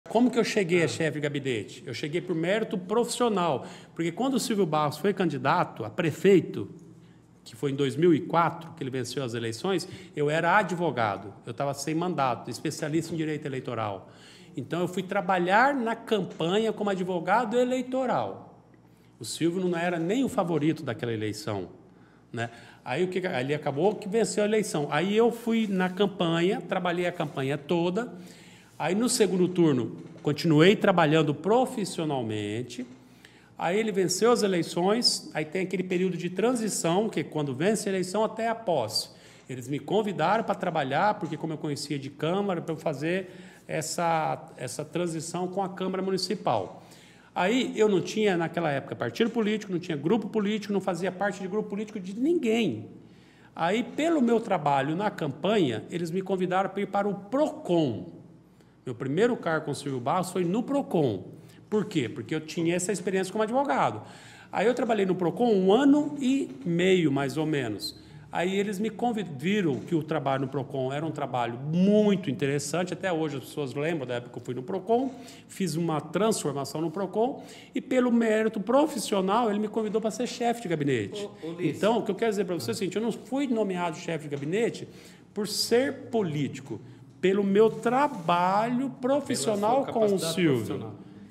Ulisses Maia fala sobre sua jornada política
No Podcast PraCima, Ulisses Maia comenta sobre como se tornou chefe de Gabinete de Maringá.